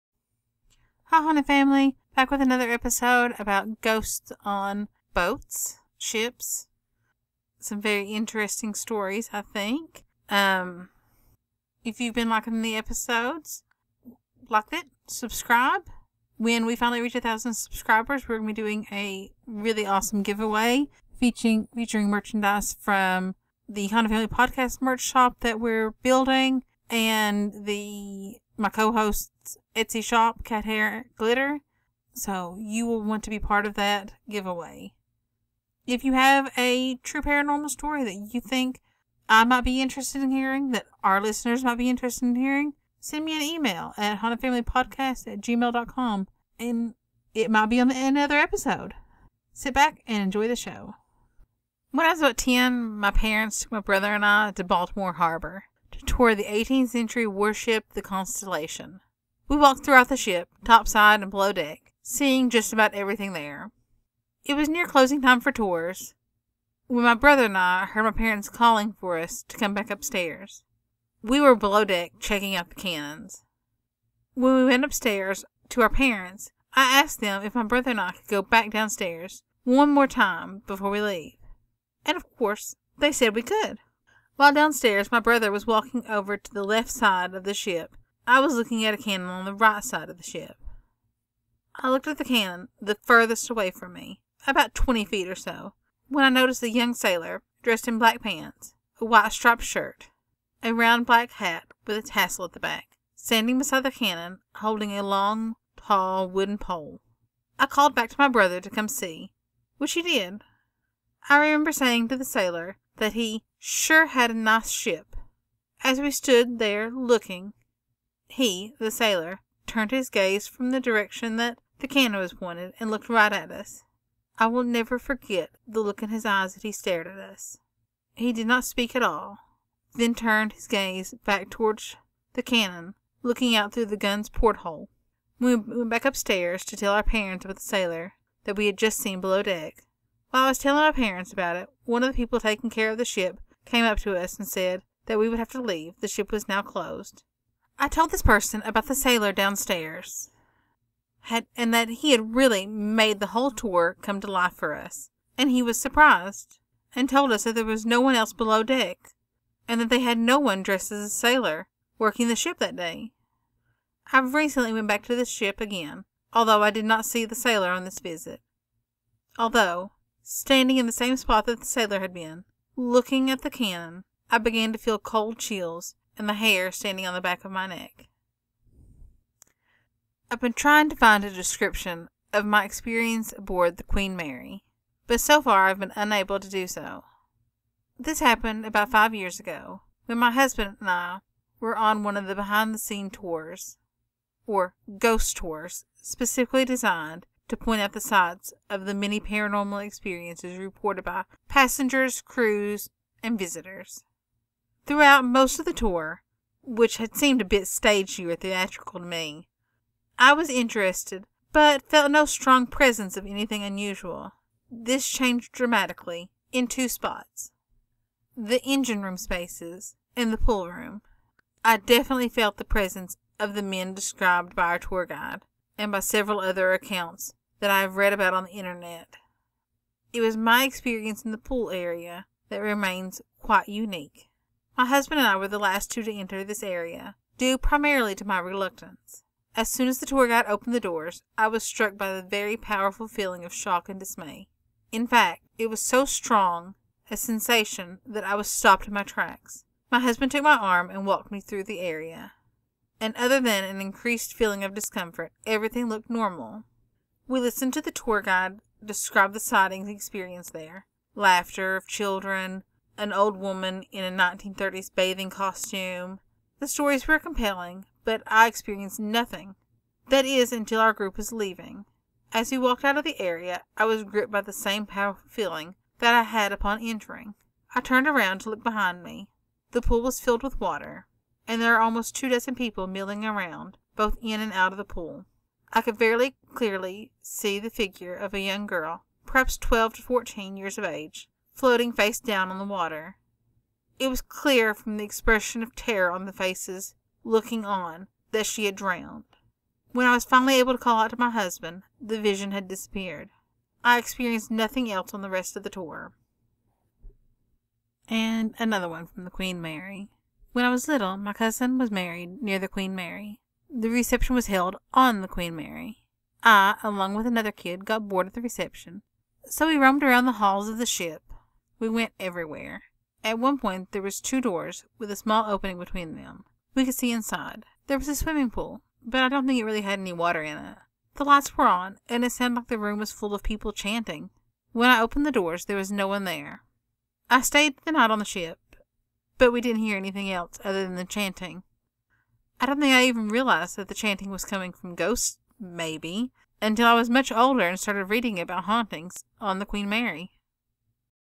True Paranormal Stories: Queen Mary
Hey Haunted Family! Welcome back for another exciting episode. This video features stories about ghostly happenings on ships, the USS Constellation and the Queen Mary.

When our little channel reaches 1000 subscribers we will have a give away!